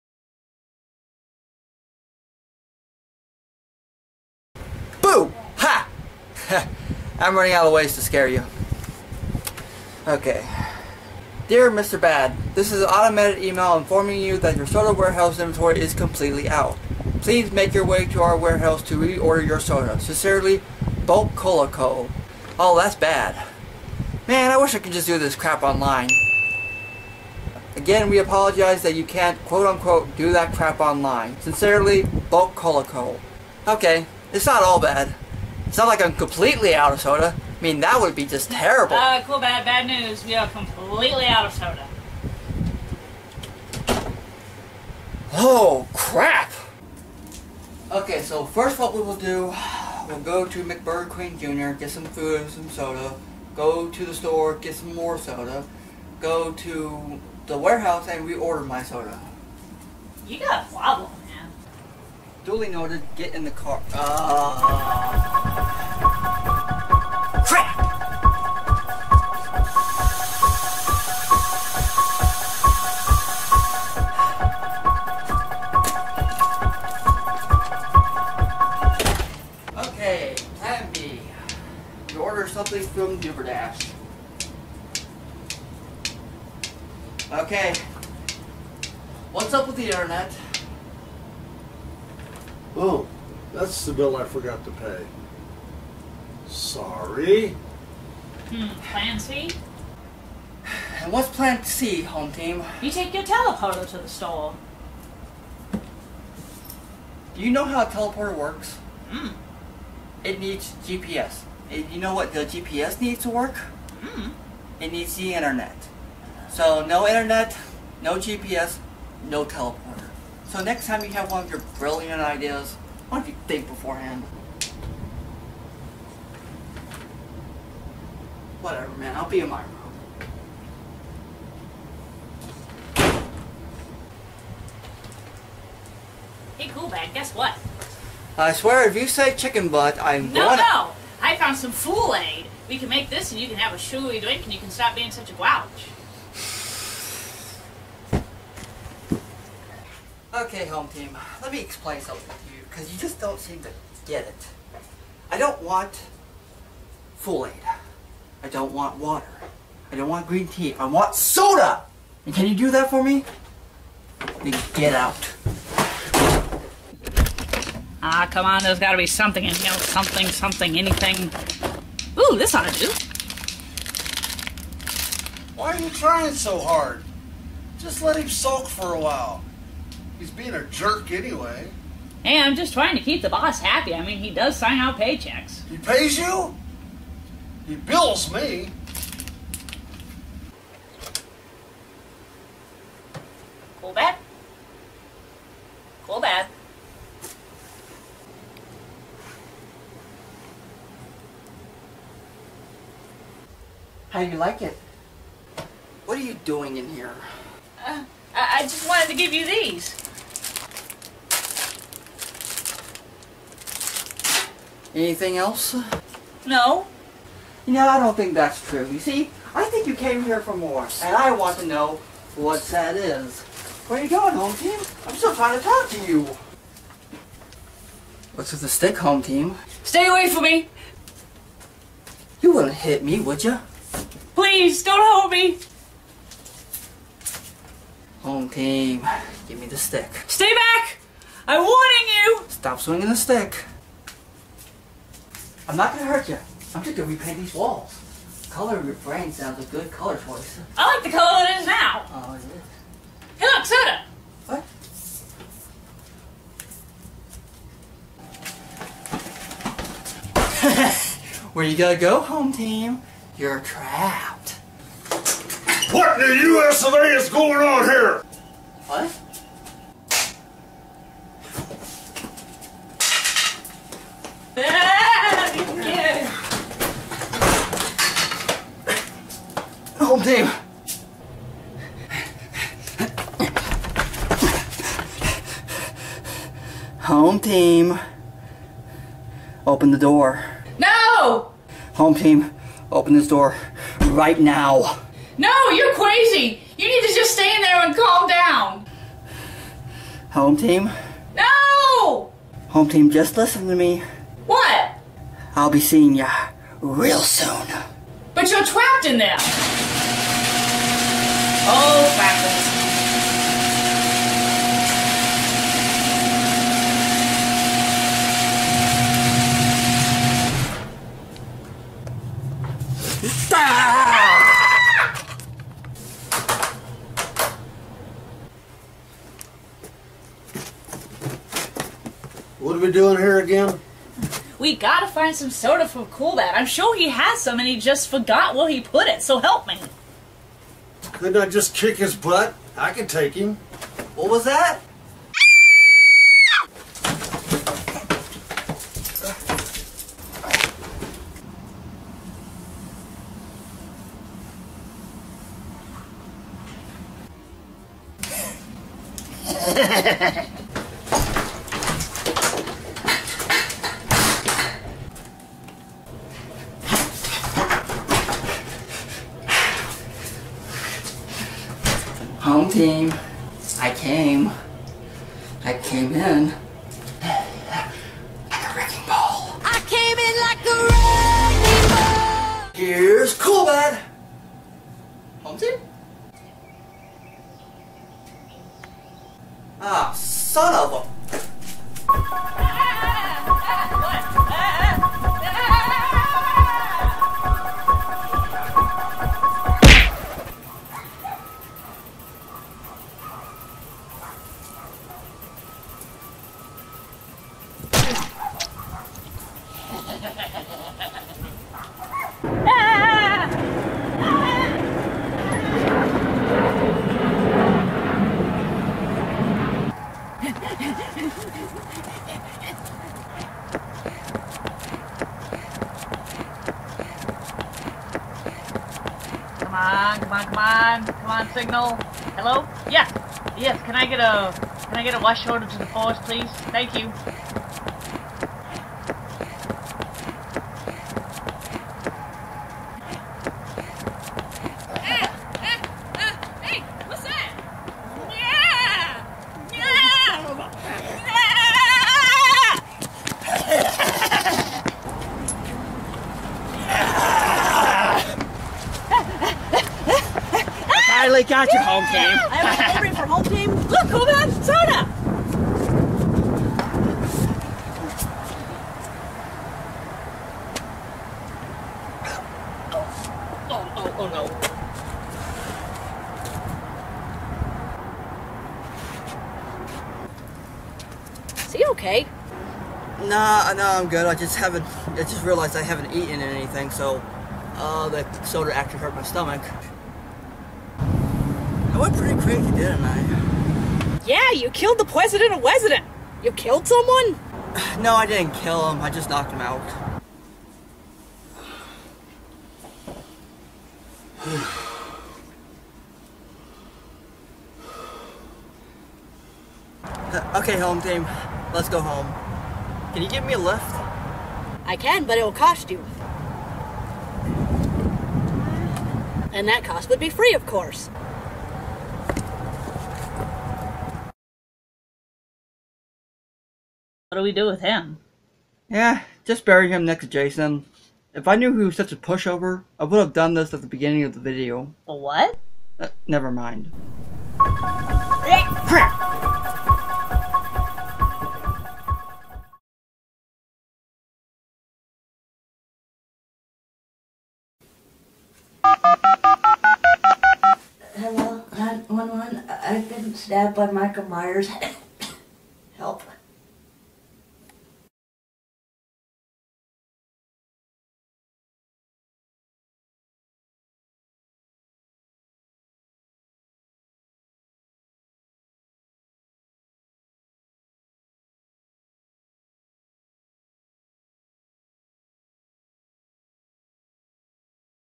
CoolBad Email #114 The Halloweening
CoolBad runs out of soda and can't get anymore because of Micheal Myers

YouTube.Com Privacy